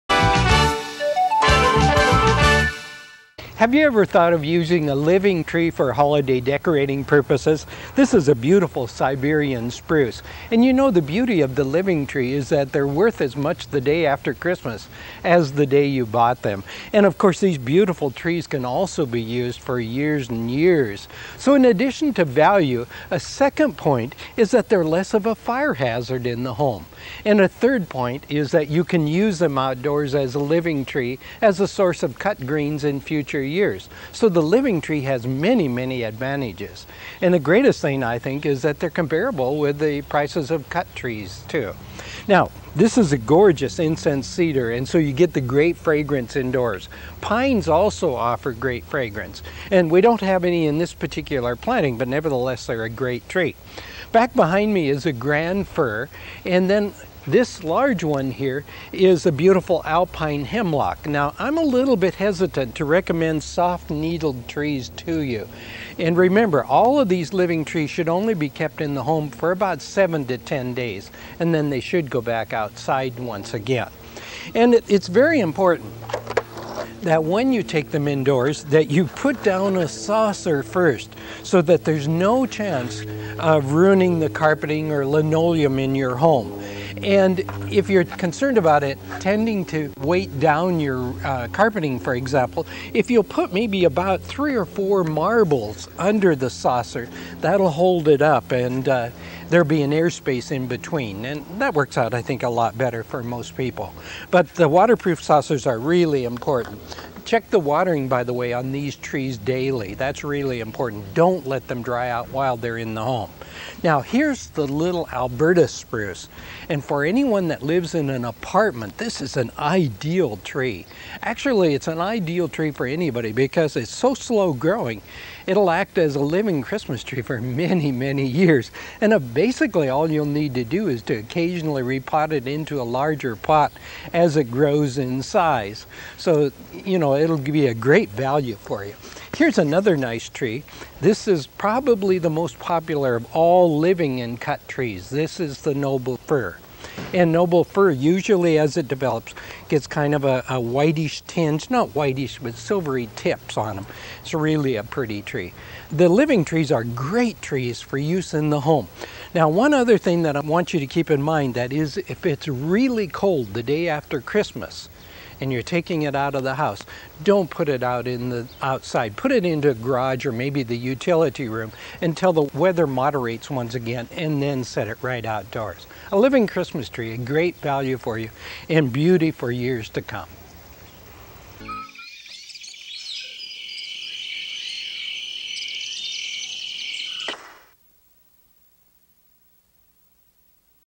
Living Christmas Trees - Gardening In America
Ed Hume discusses how to choose and care for living Christmas trees.